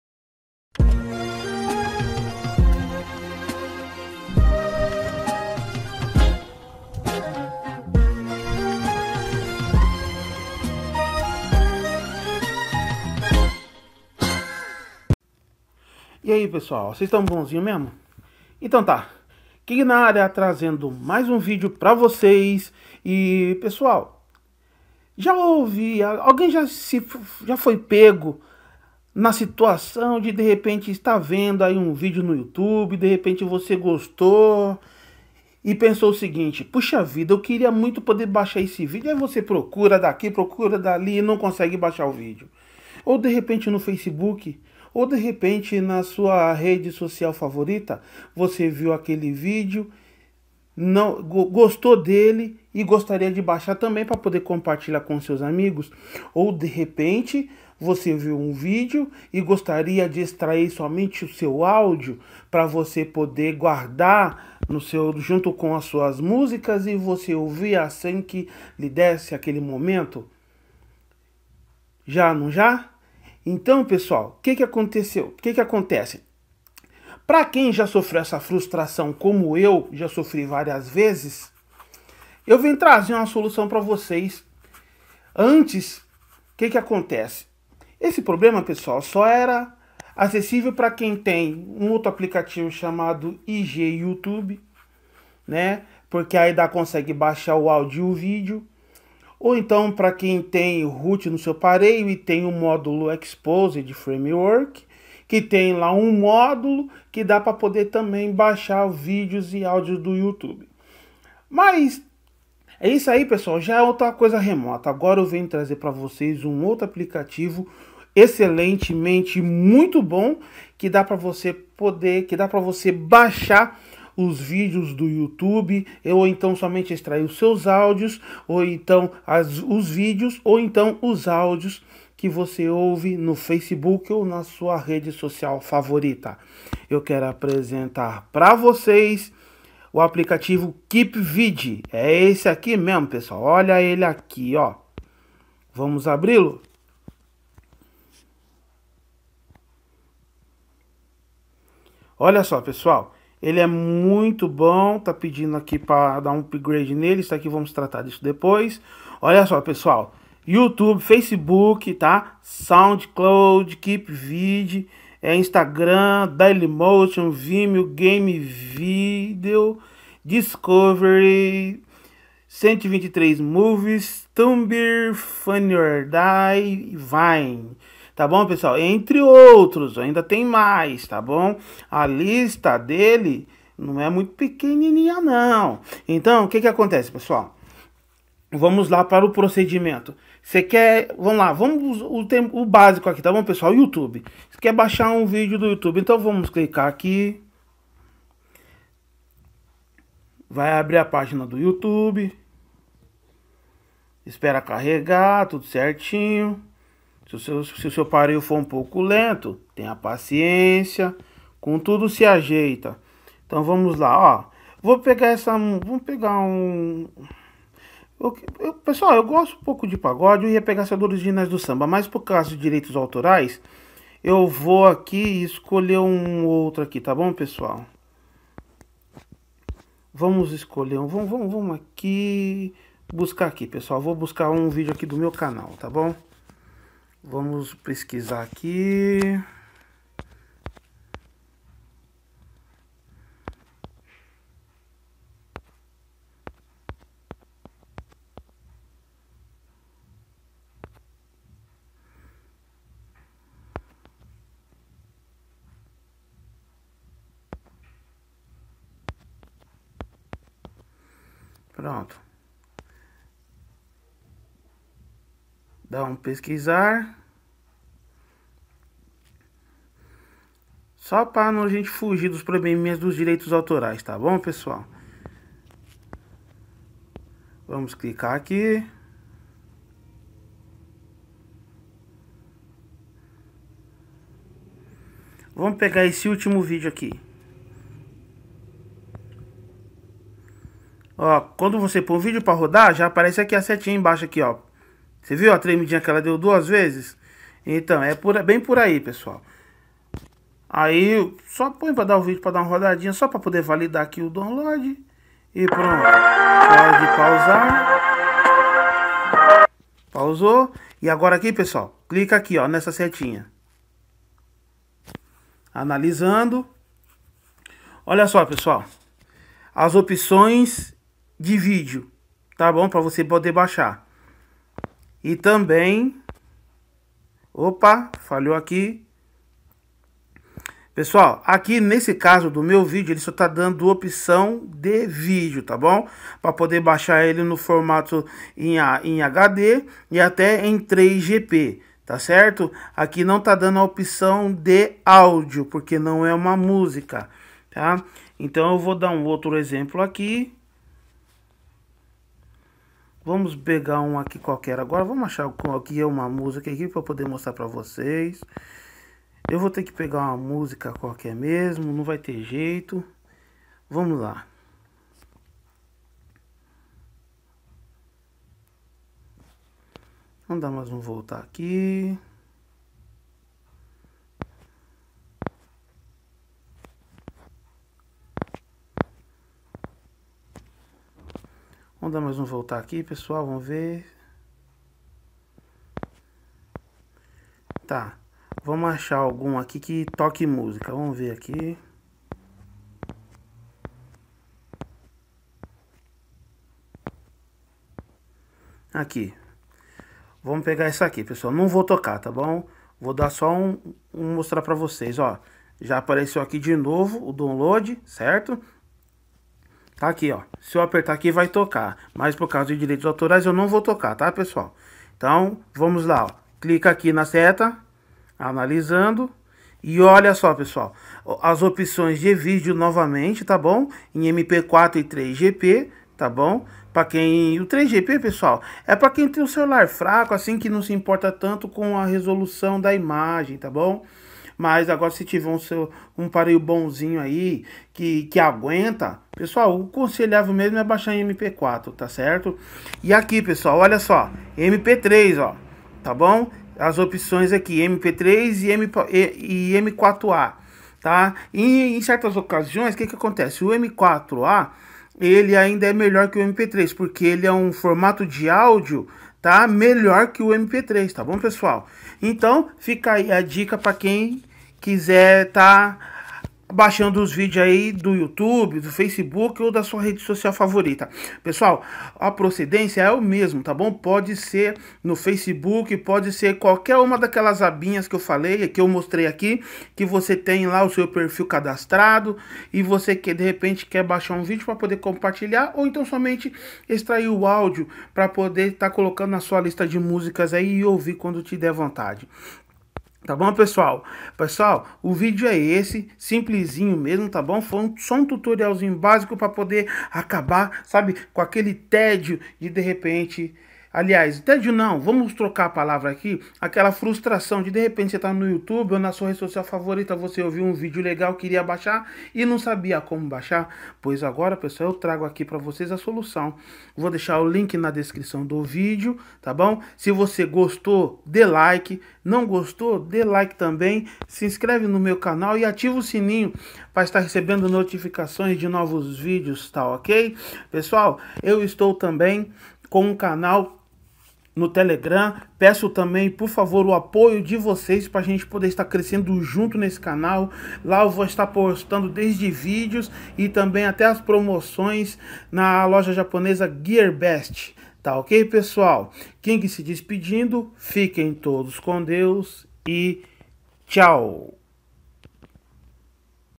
KeepVid 2017 - Baixe Vídeos e Áudios das suas Redes Sociais Favoritas
Olá pessoal neste vídeo vou mostrar à vocês que é possível baixar áudio e vídeo do YouTube ou de sua rede social favorita sem precisão de root, aproveitem o vídeo e não se esqueçam do like👍.
▬▬▬▬▬▬▬▬▬▬▬ஜ۩۞۩ஜ▬▬▬▬▬▬▬▬▬▬▬▬▬
▓▓▓▒▒▒░░░ LEIA A DESCRIÇÃO DO VÍDEO ░░░▒▒▒▓▓▓
▬▬▬▬▬▬▬▬▬▬▬ஜ۩۞۩ஜ▬▬▬▬▬▬▬▬▬▬▬▬▬

Parceiros:👍
xxxxxxxxxxxxxxxxxxxxxx